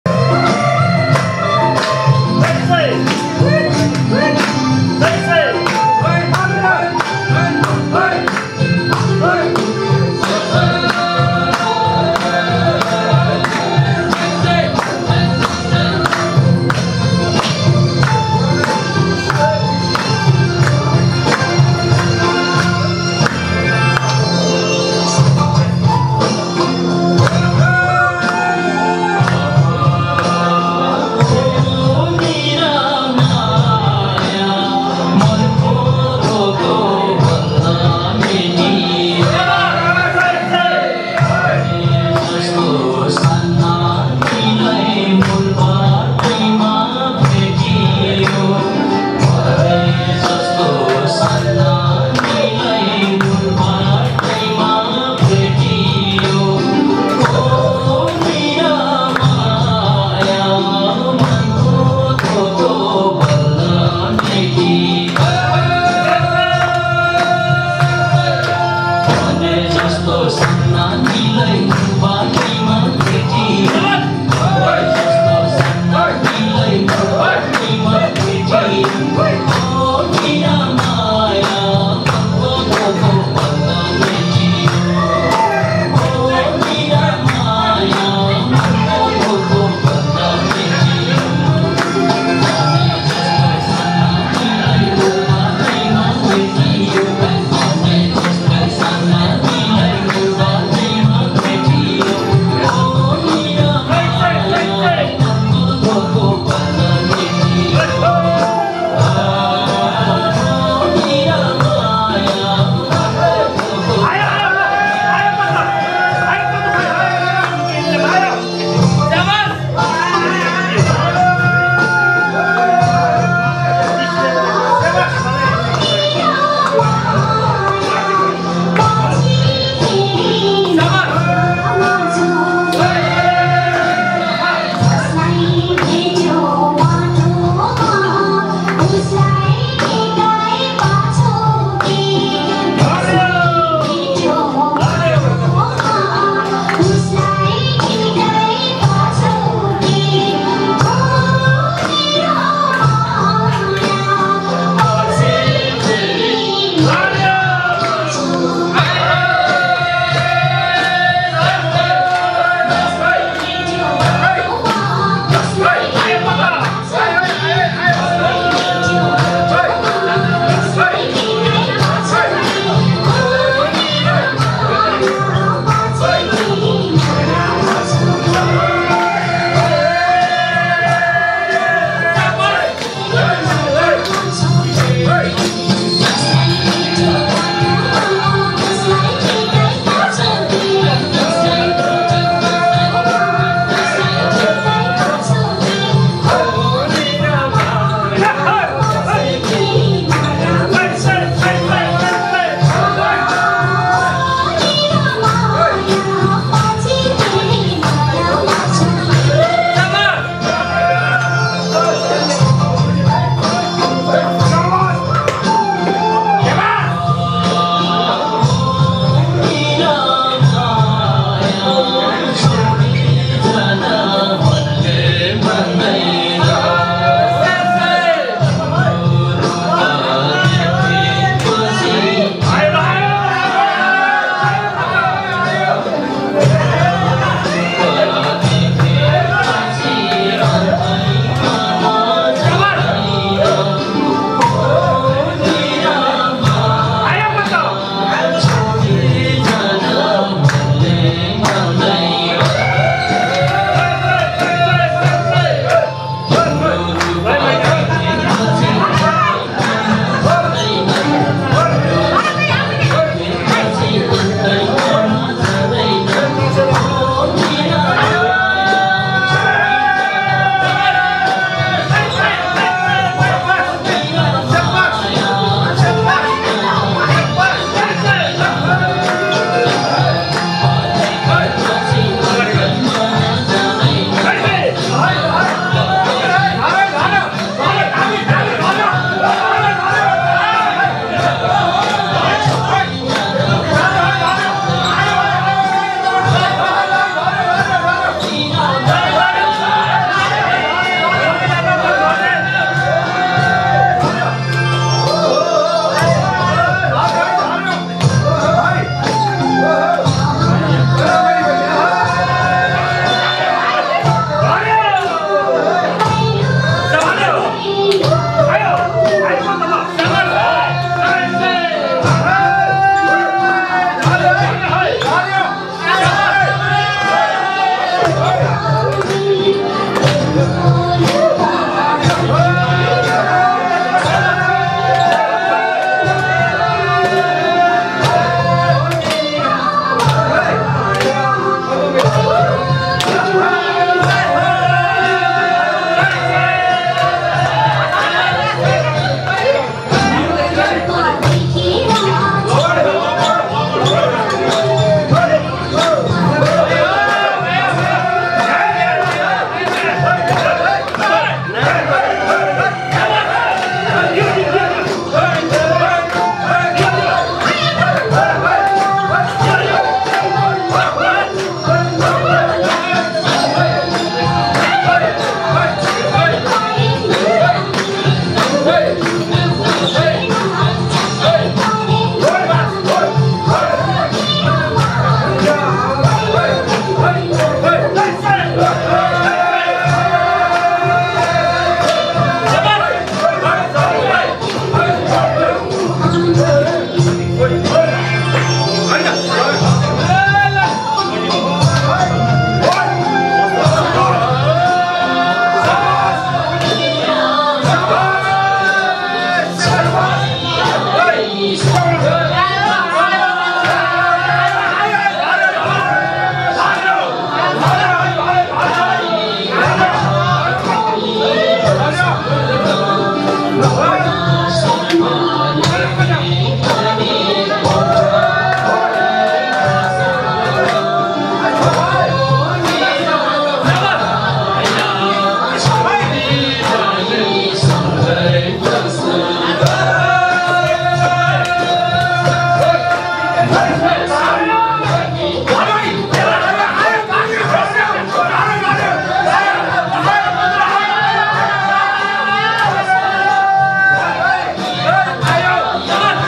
new nepali kauda song live perfomance magar sangh in macau maghe celebrating
organizer magar assocation of macau china sar